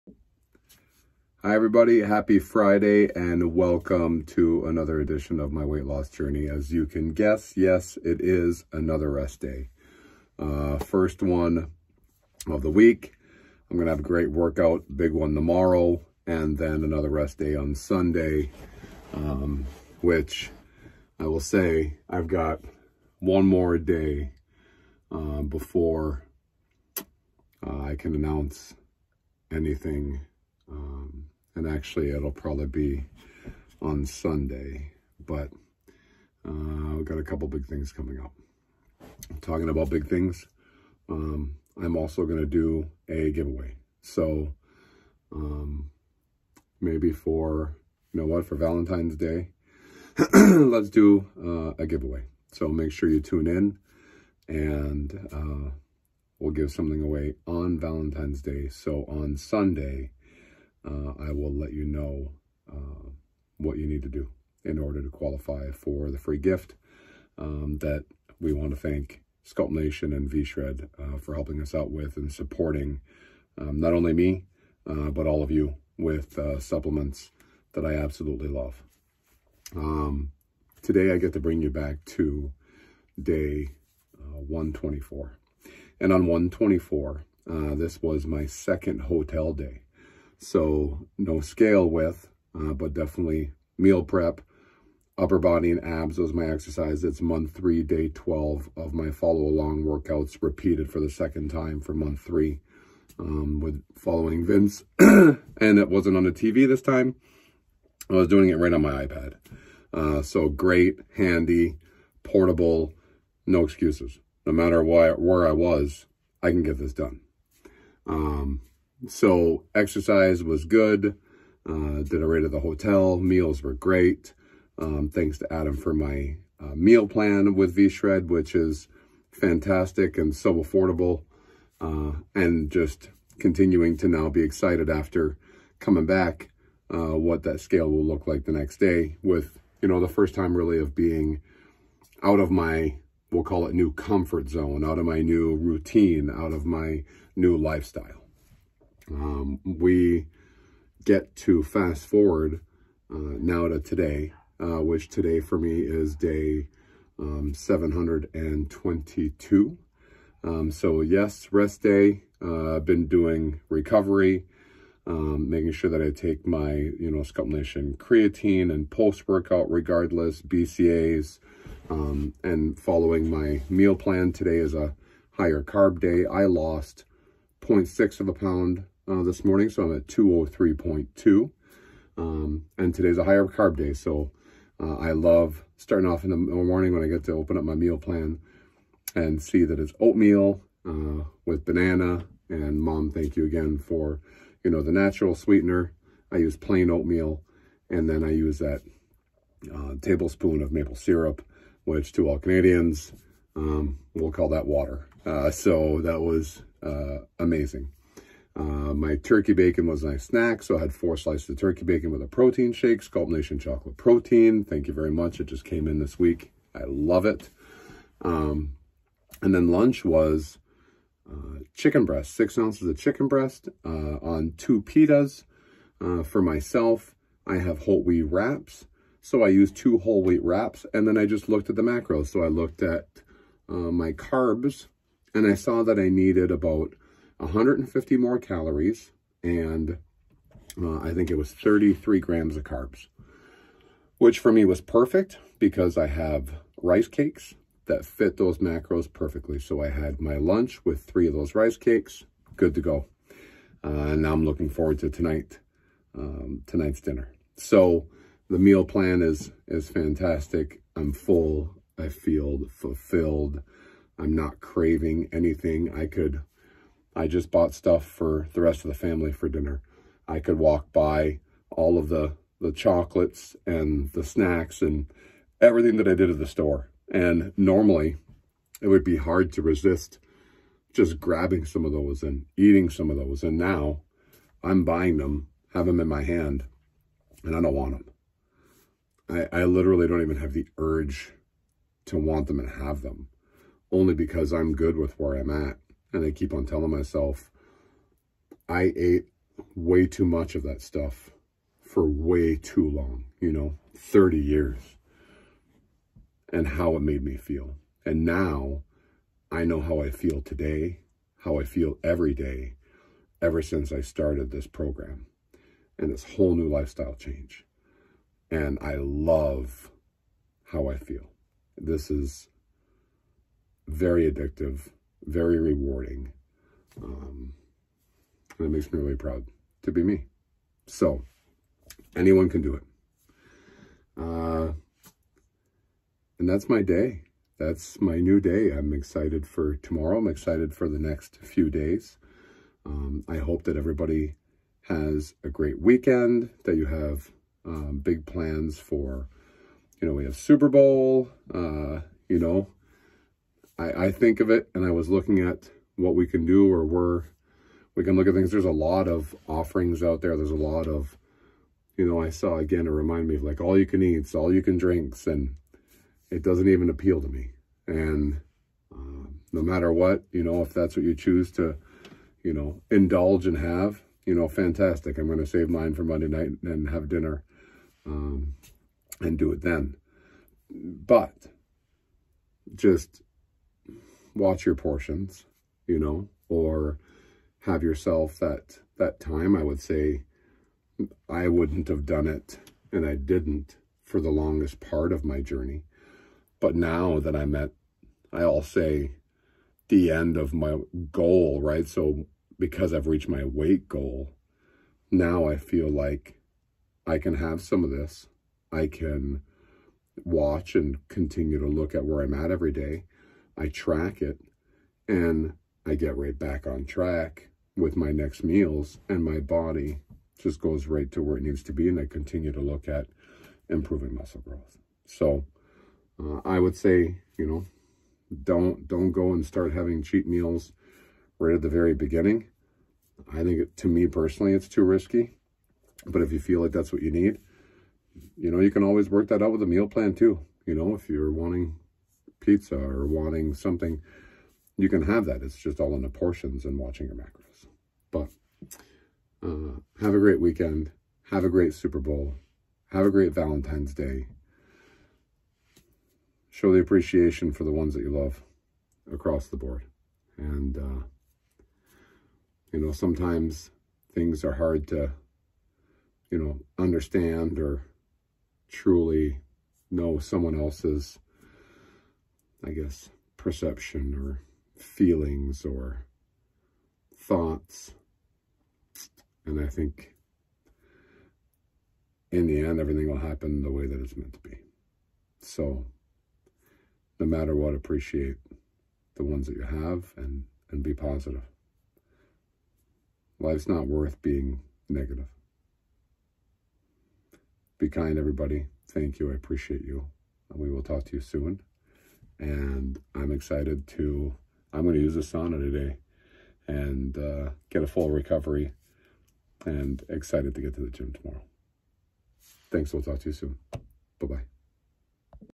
My Weight Loss Journey
Day 124
Month 3 Day 12 of the FLE follow along workouts.
Repeating month 3 for the second time.

Upper Body and Abs

No weight today with being in a hotel for the 2nd day.

VShred Meal Plan Day 5

Day 722

Month 2 Day 12 of the VShred Big Arms Program.
Rest Day

VShred Meal Plan Day 3

-.6lbs for a current weight of 203.2lbs.

Believe in yourself. Trust yourself and set yourself up right. In the end, you will win.

Be positive and kind!